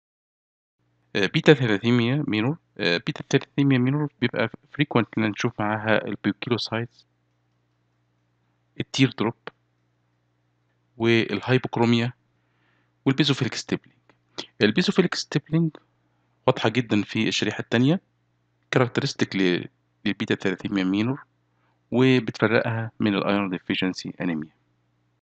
Hematology - 2 | U1-L36| Beta thalassemia minor
Lecture - 36 : Beta thalassemia minor
1- Poikilocytes
2- Tear drops
3- Basophilic stippling